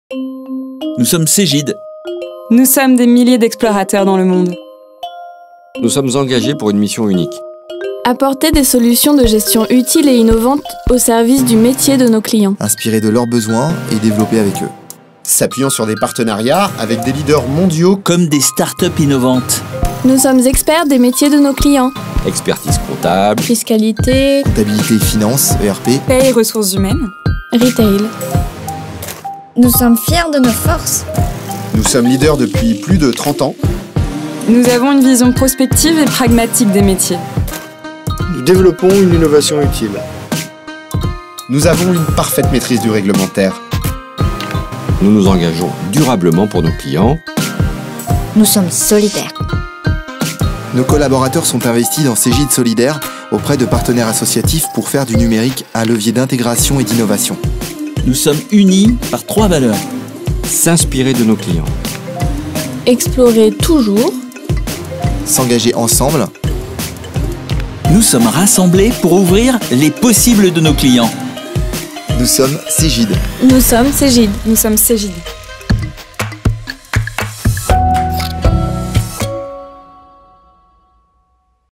Qu'est-ce que Cegid ?
Depuis plus de 30 ans, Cegid crée des solutions de gestion utiles et innovantes au service du métier de ses clients : Expertise Comptable, Ressources Humaines, Paie, Retail, Fiscalité, ERP.

Qui sommes-nous ?

Spécialisés dans l'édition de logiciels de gestion pour les professionnels, nous aidons au quotidien les experts-comptables, les professionnels des ressources humaines, de la finance et de l’ERP, de la fiscalité et du retail à atteindre leurs objectifs.

Sur cette chaîne, vous trouverez des démos produits, les témoignages de nos clients mais aussi des avis d’experts sur les tendances de demain.